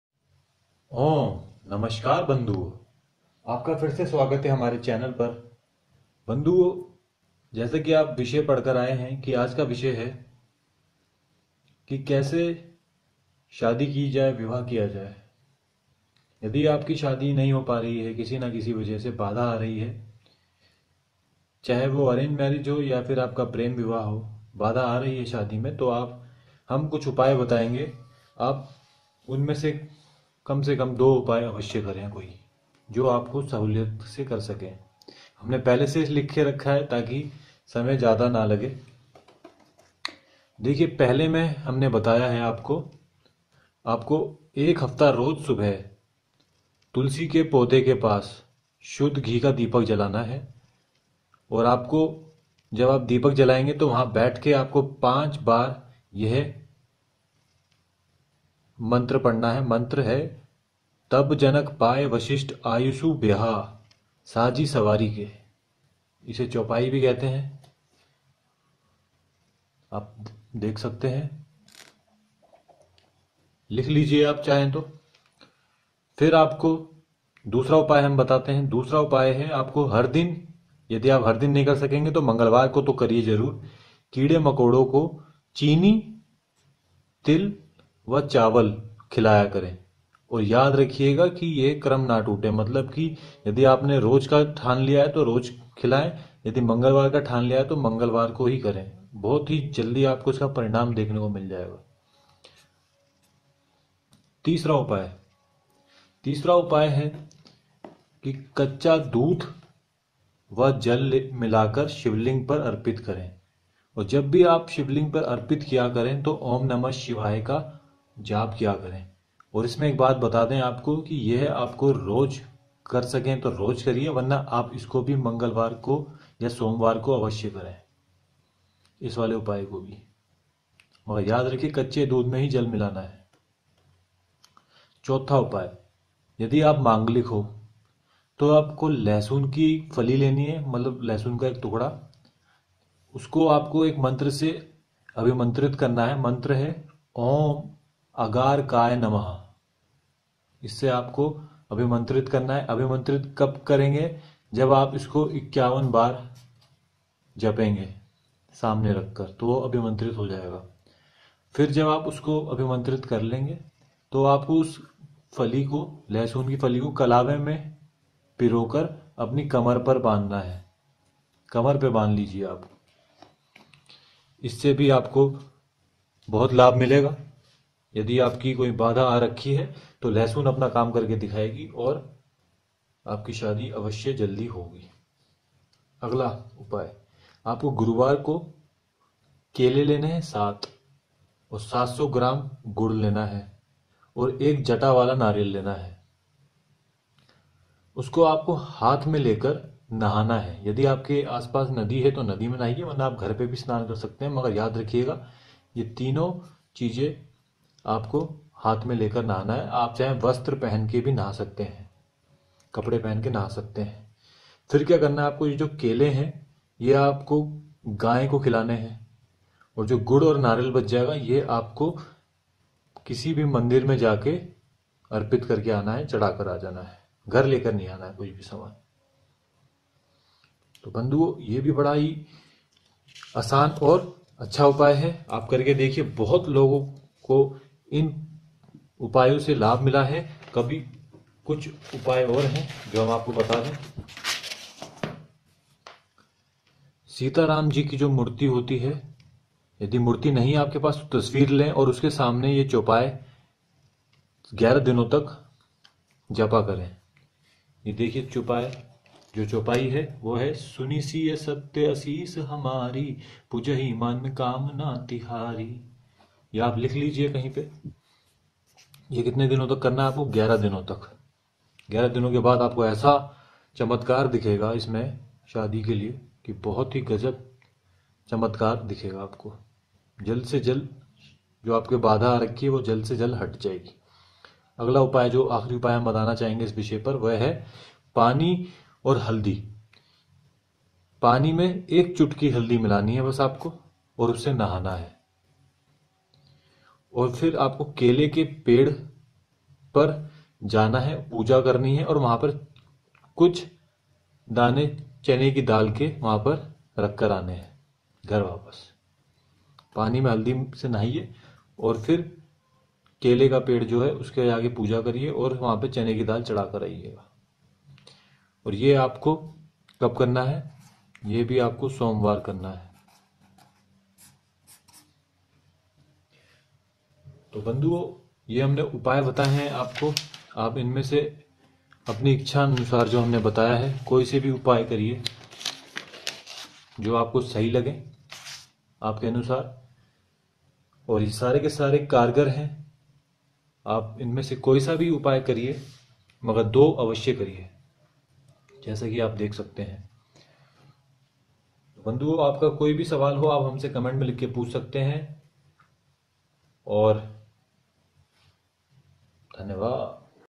SHADI karne ke liye SATEEK TOTKE शादी विवाह ना हो पा रही हो तो करिए ये उपाए
is video mai aapko bataya jayega ki kaise aap apni shadi mai aaye badhao ko hata sakte hai or uchit samaye par shadi kar sakte hai yadi aapki shadi nahiho paa rahi ho kisi na kisi wajah se to aap in 7 upaayo mai se koi bhi 2 upaye karke dekhiye aapko niraasha hath nahi lagegi
we will not be responsible if you will be unable to achieve your goal after prayog, sadhna, totka ,  nushkha .